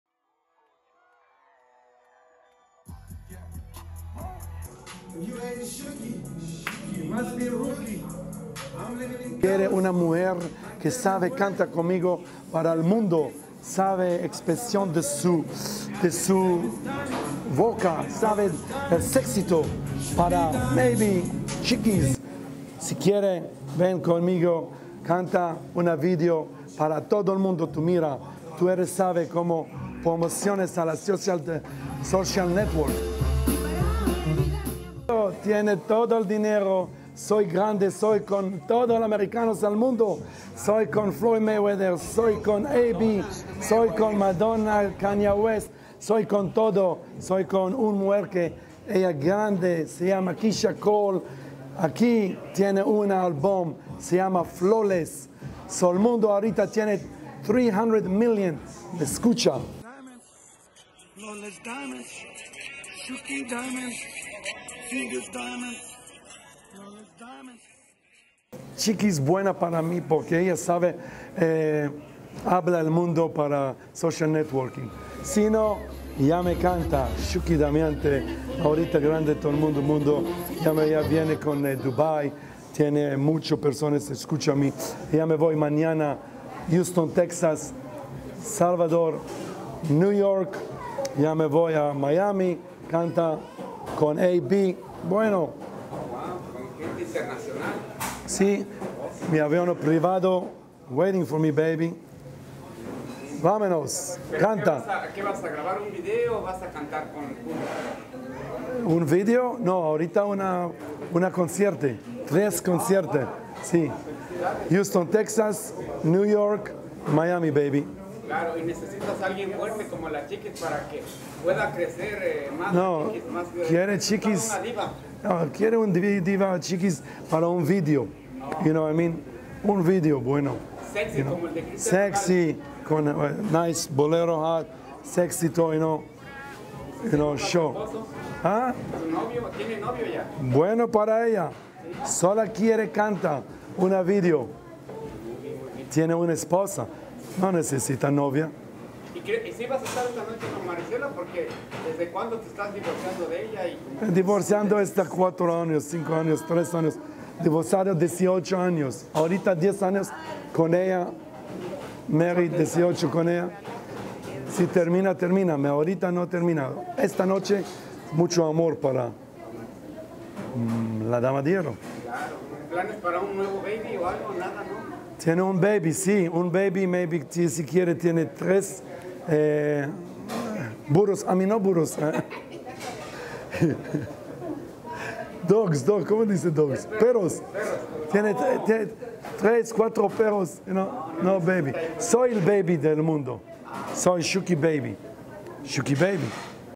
CHIQUIS RIVERA RECIBE PROPUESTA MILLONARIA
CHIQUIS RIVERA FAMOSO SULTAN  SHUKI  OFRECE PROPUESTA MILLONARIA PARA UN
VIDEO MUSICAL CON EL SULTAN !
RIVERAS # CHIQUIS # JENNI # LUPILLO R # DINASTIA RIVERA # LA DIVA DE LA BANDA #
REGIONAL M #